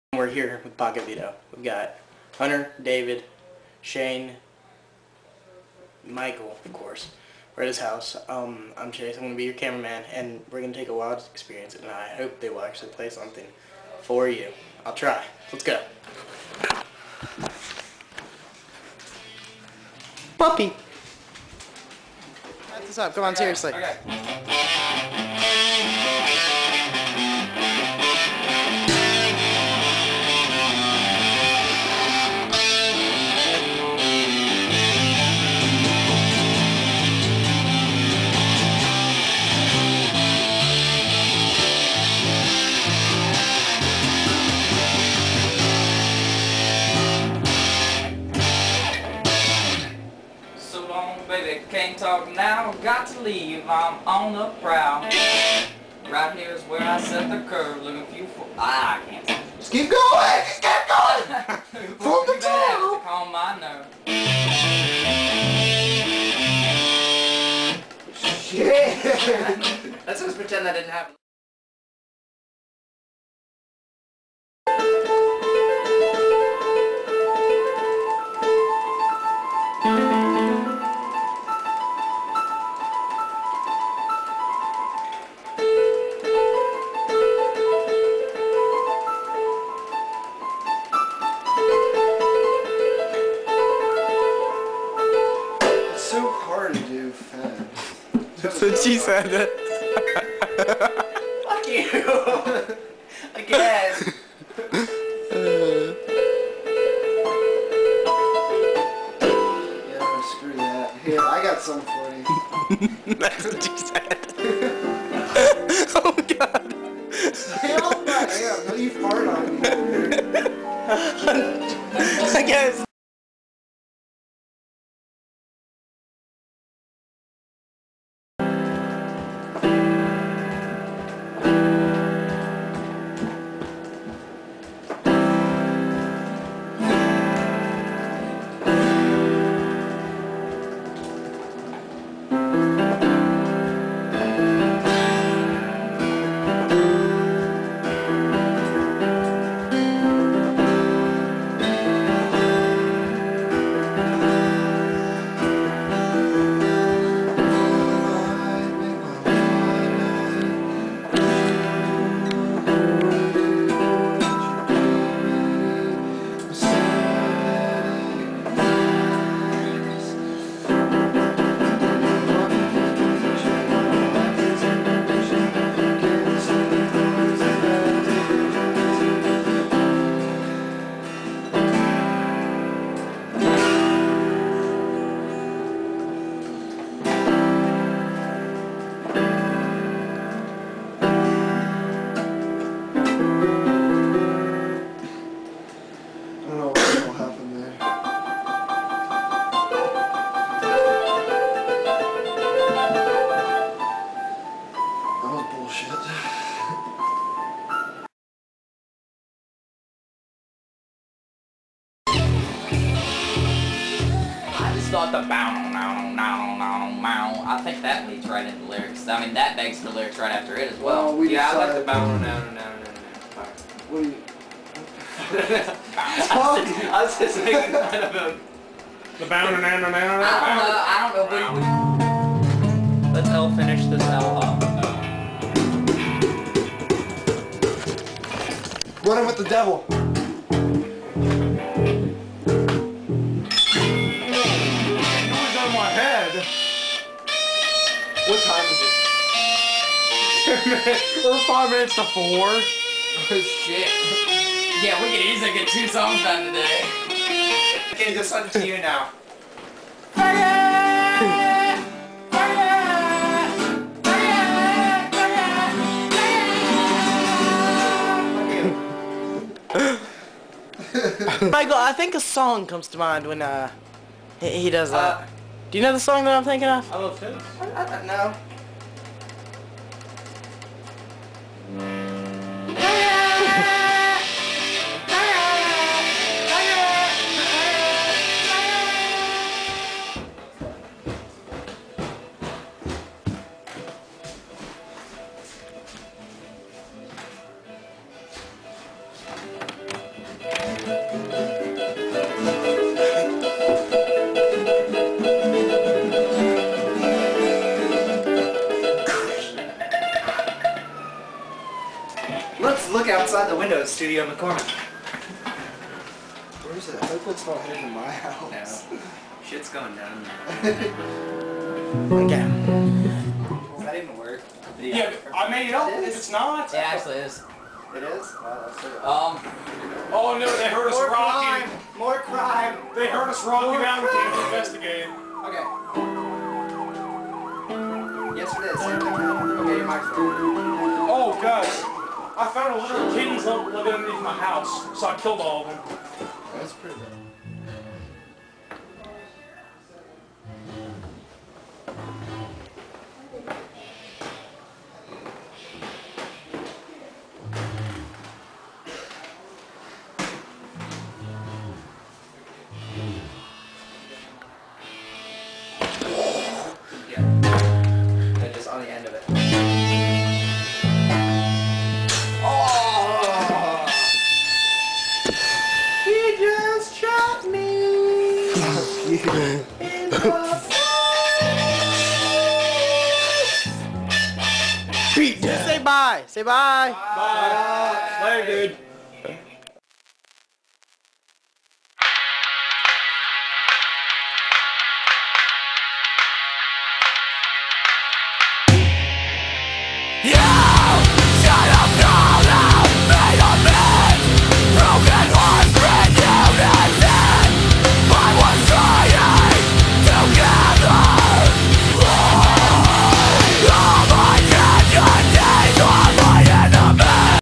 Pocket Veto Practice Montage
This a quick little view at what the sprouting band, pocket veto, is up to.